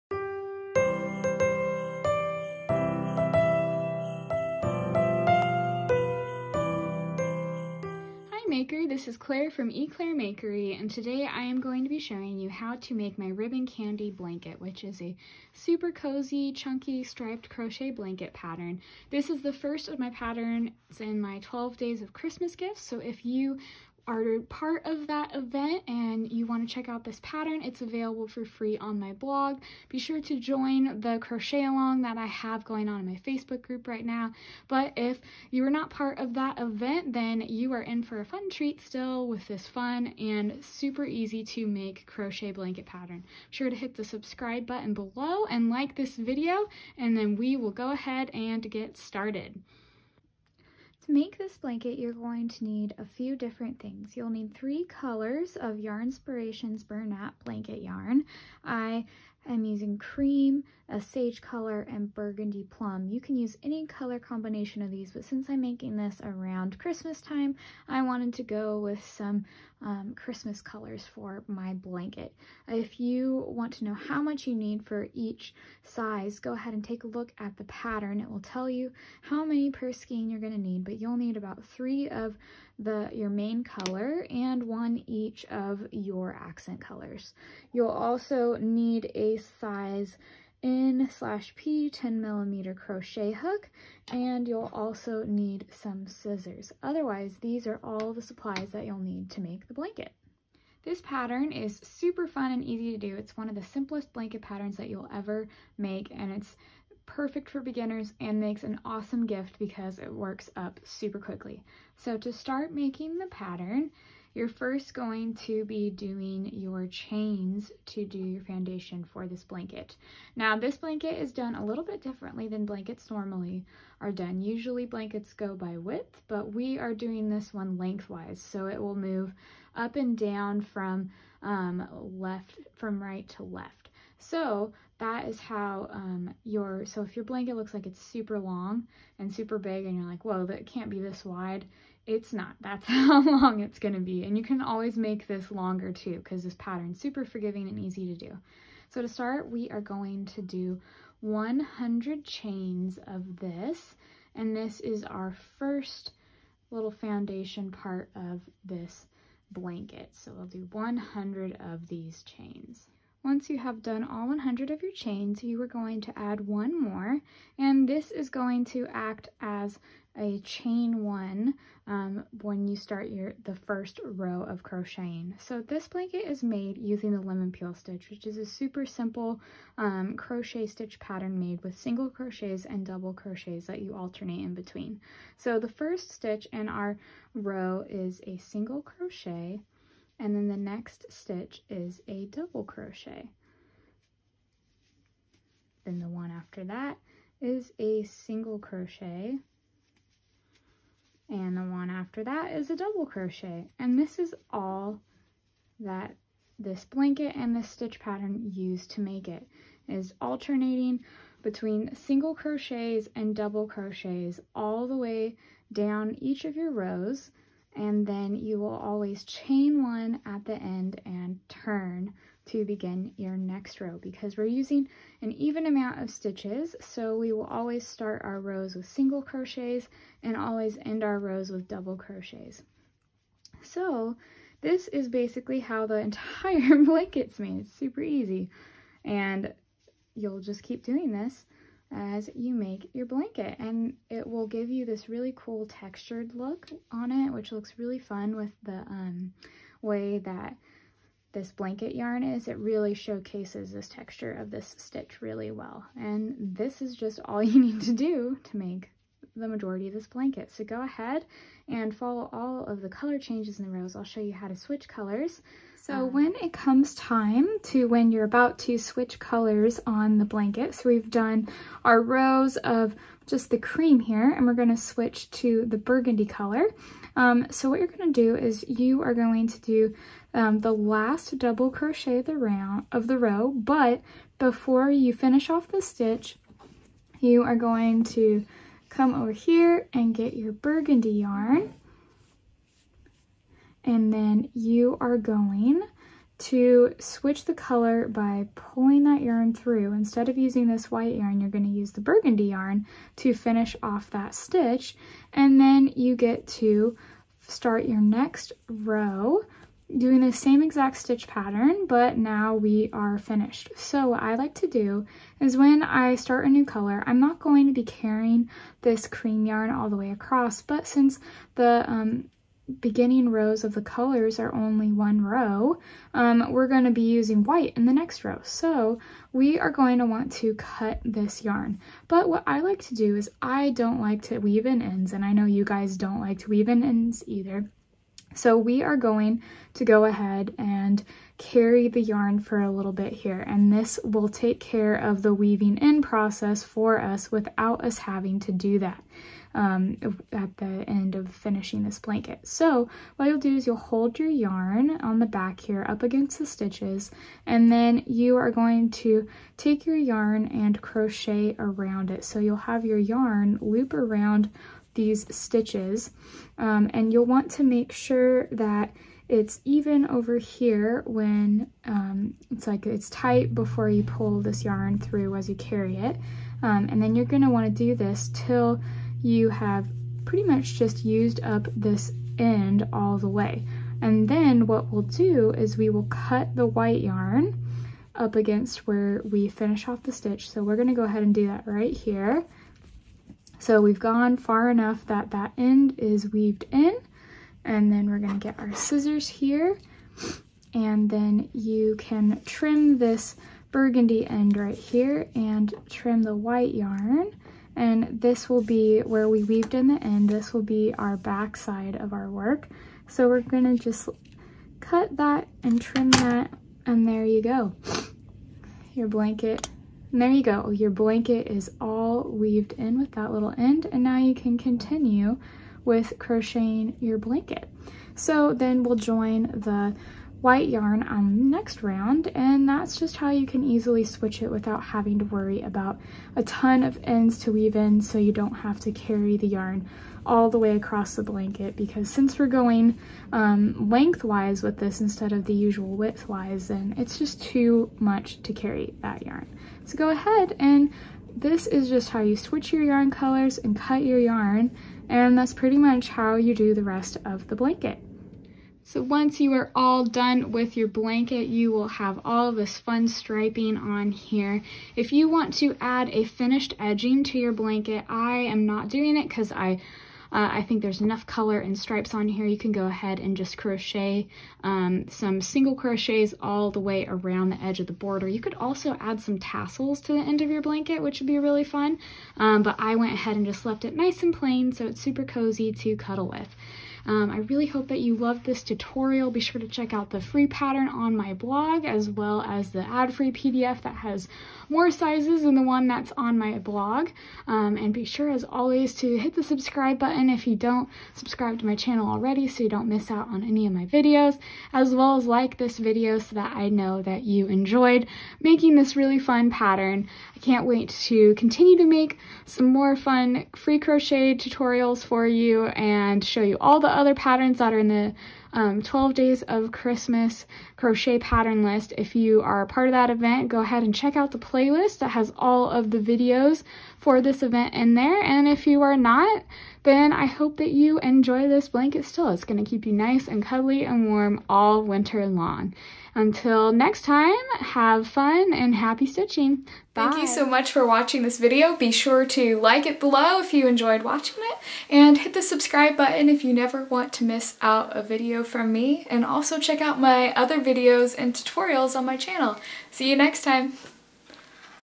How to Crochet the Ribbon Candy Blanket: Free Chunky Crochet Blanket Pattern and Tutorial
Learn how to crochet this festive quick and easy chunky crochet blanket pattern, the Ribbon Candy Blanket. This striped chunky blanket is made with Bernat Blanket Yarn using the lemon peel stitch, and is super cozy.